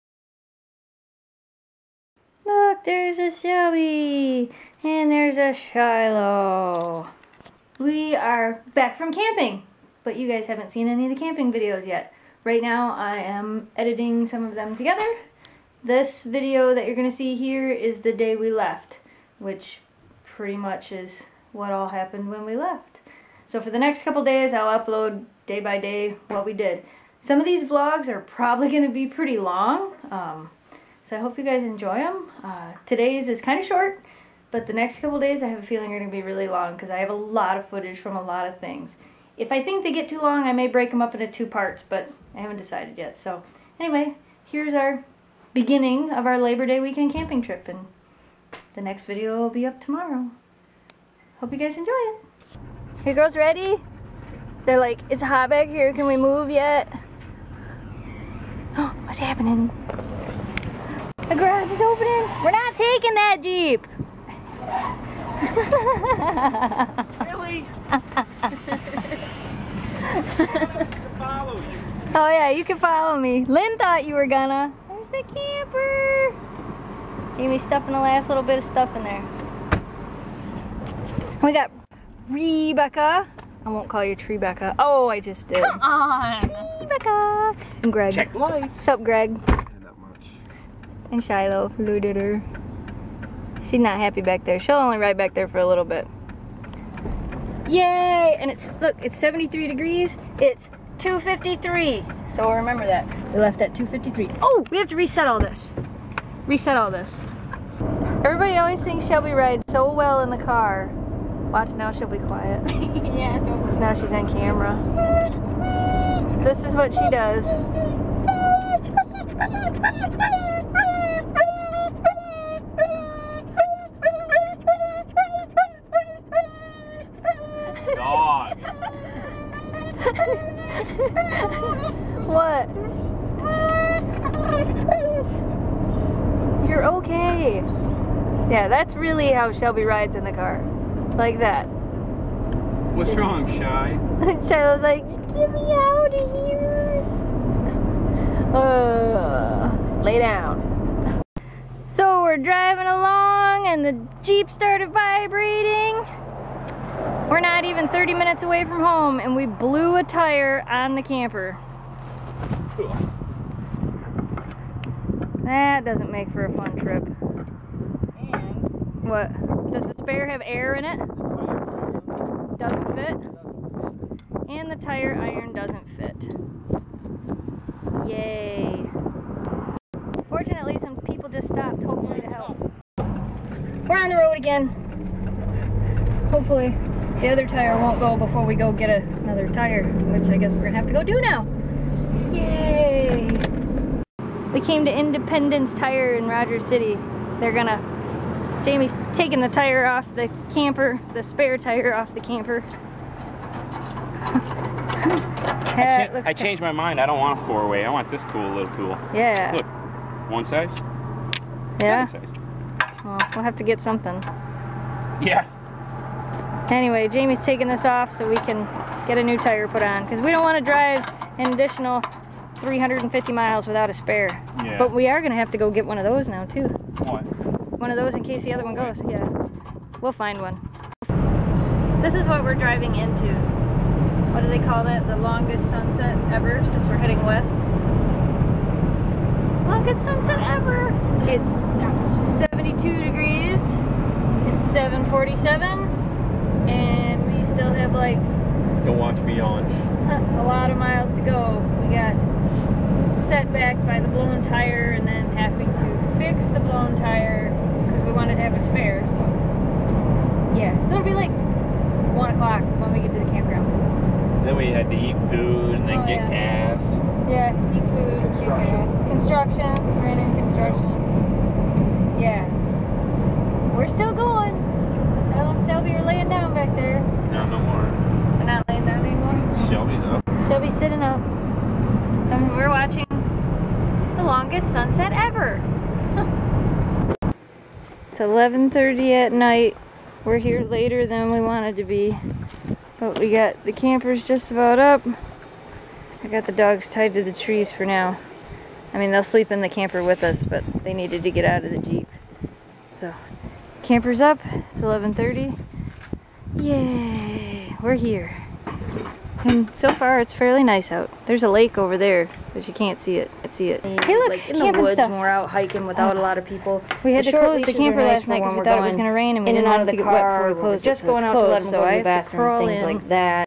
Blown Tire on the Camper, Day 1 Camping Trip 9/1/11
Yeah so day 1. . we left Alpena at 3pm, got 26 miles from home and blew a tire on the camper, that set us back over an hour.  But we continued onward!